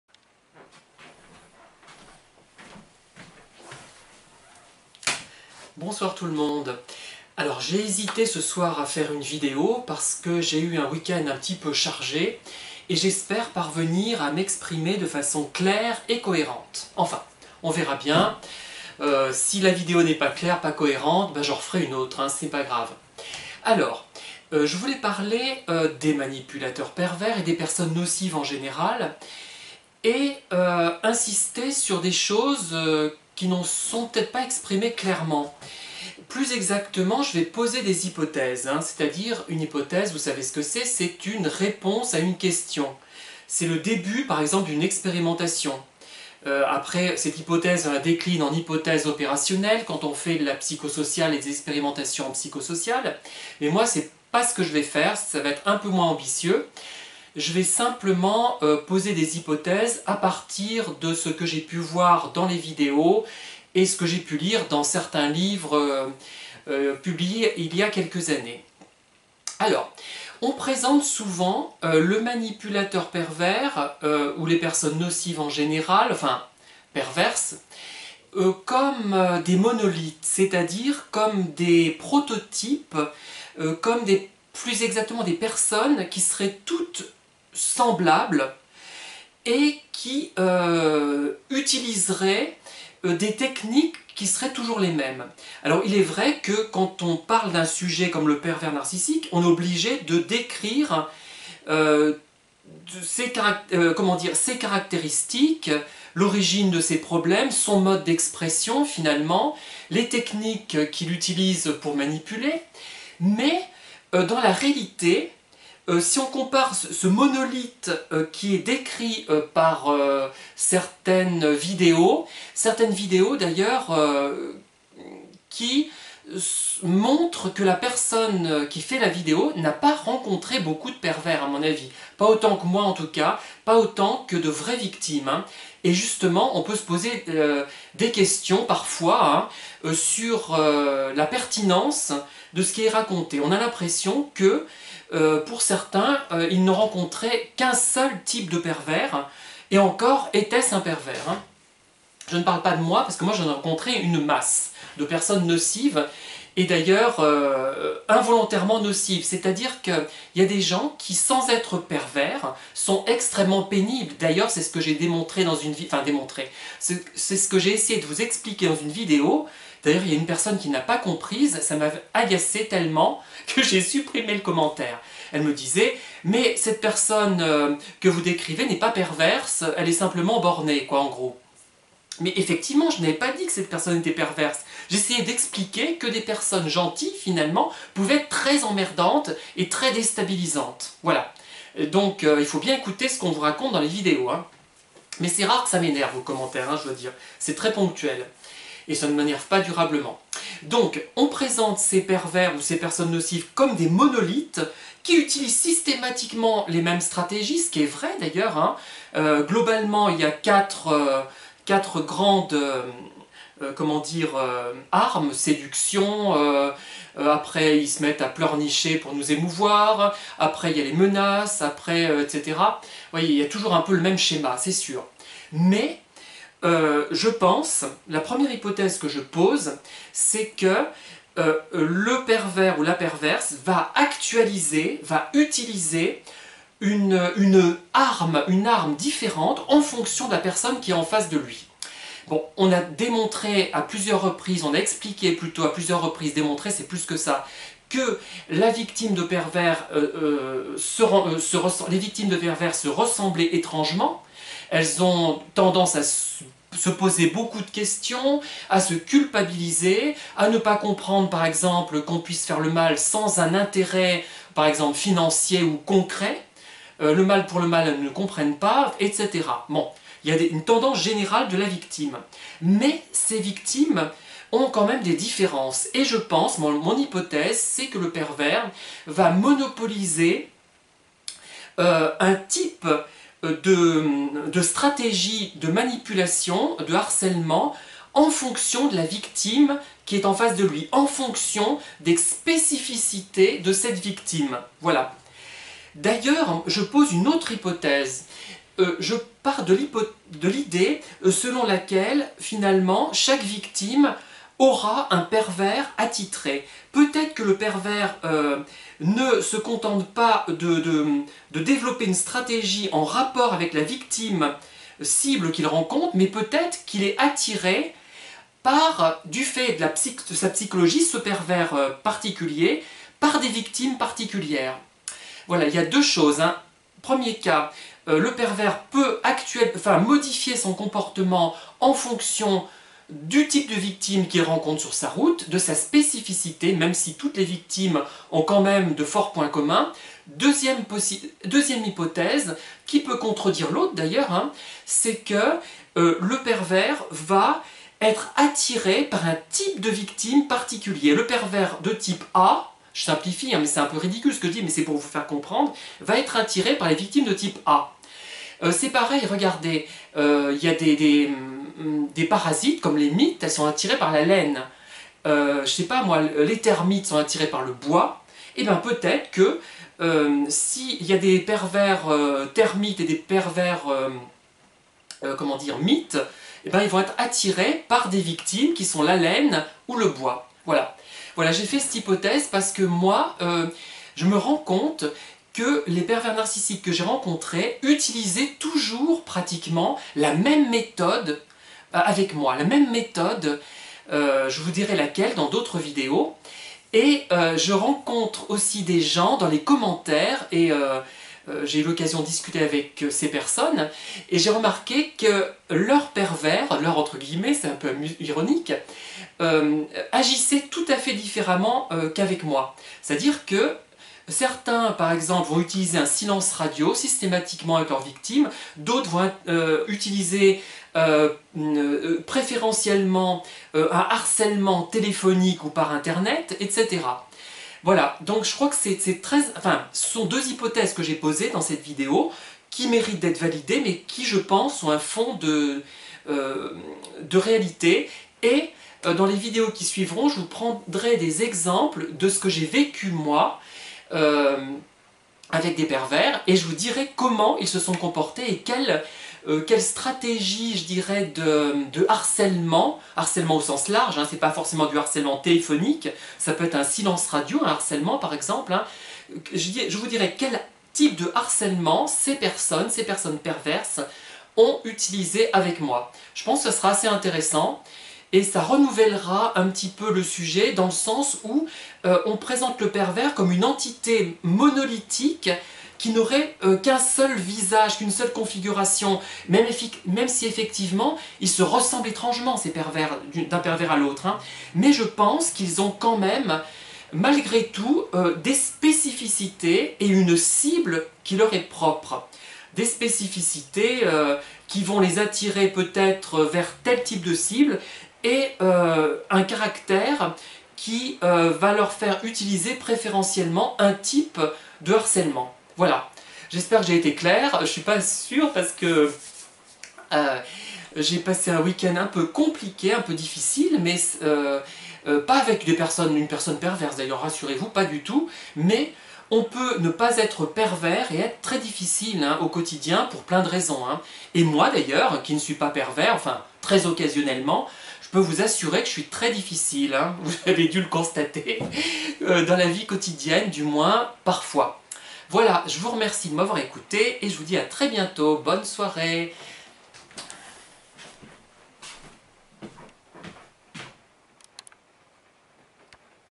Le pervers narcissique s'adapte-t-il à sa victime ou est-il conçu pour un certain type de victime ?
Deux hypthèses sont posées dans cette vidéo :
1 - Le pervers narcissique adapte-t-il une stratégie de harcèlement qui varie en fonction de la victime que le hasard lui fait rencontrer?
2 - Chaque pervers narcissique a-t-il un type de victime préférentiel?